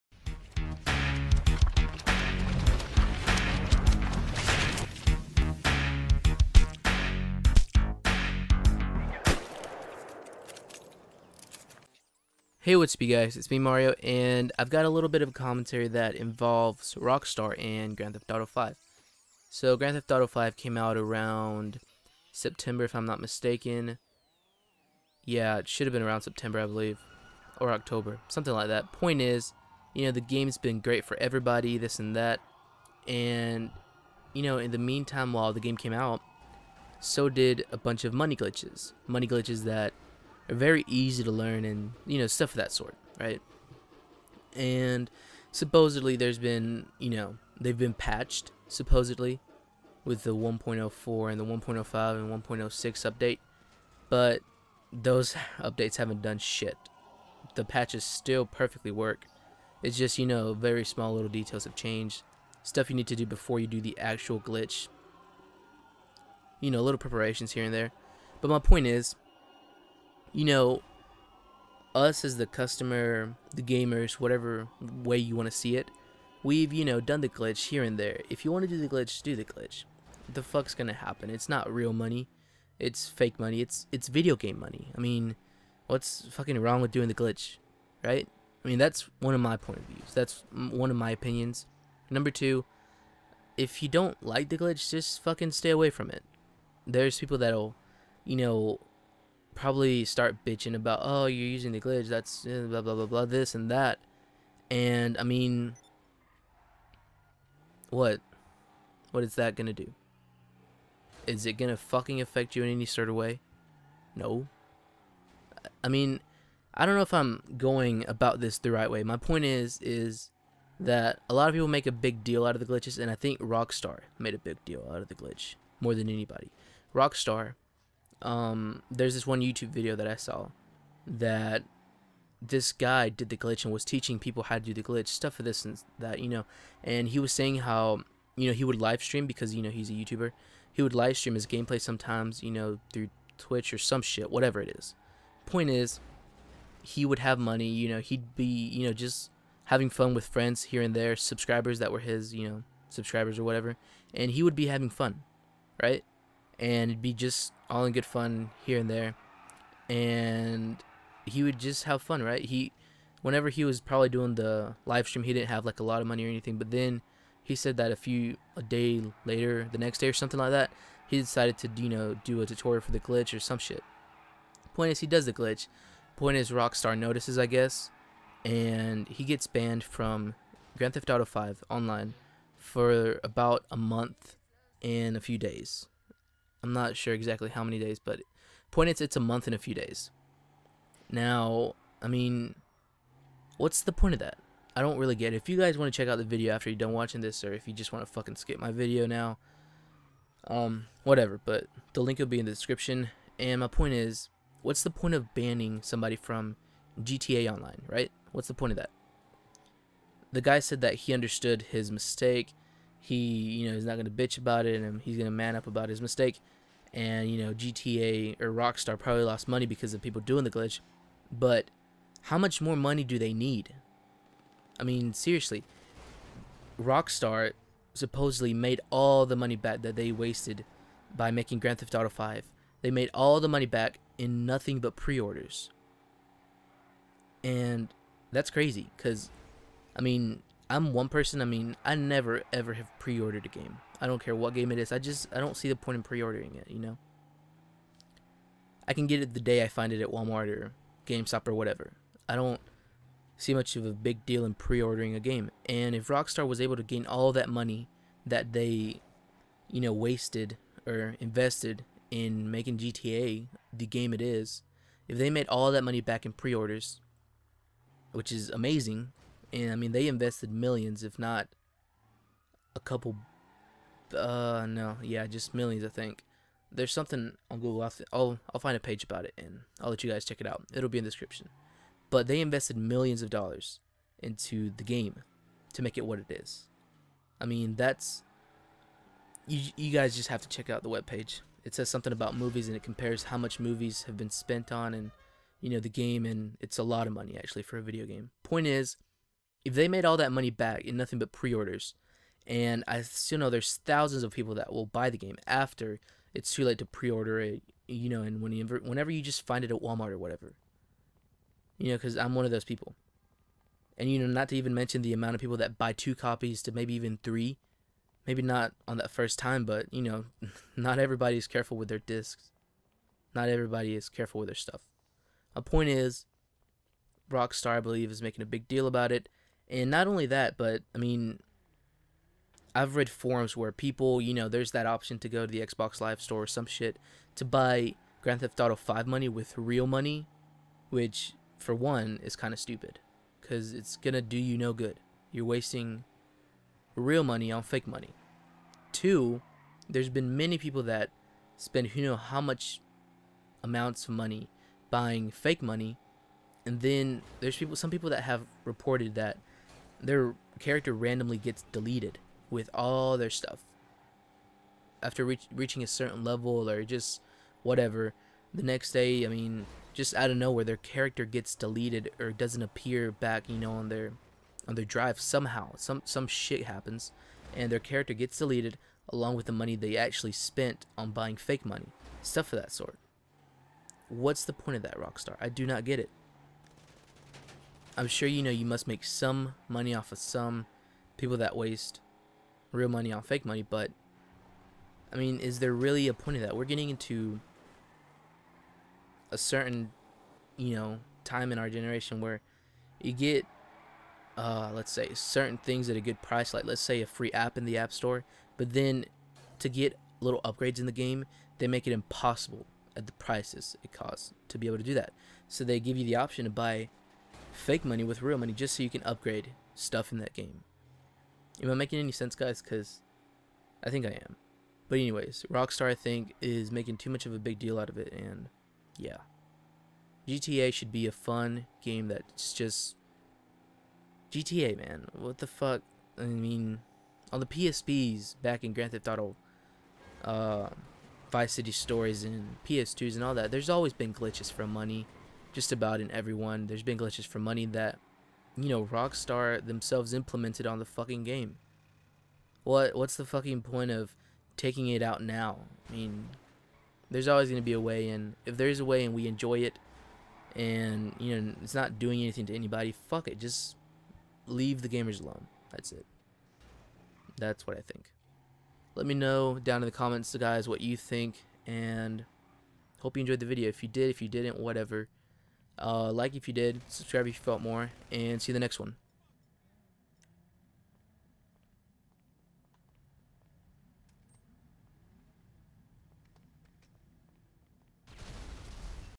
GTA 5 Rockstar Rant - Banned For Money Glitch
so its just a bitt of a rant

This isn't fair ! this guy got banned again for no reason!

Intro template by FocusCreations.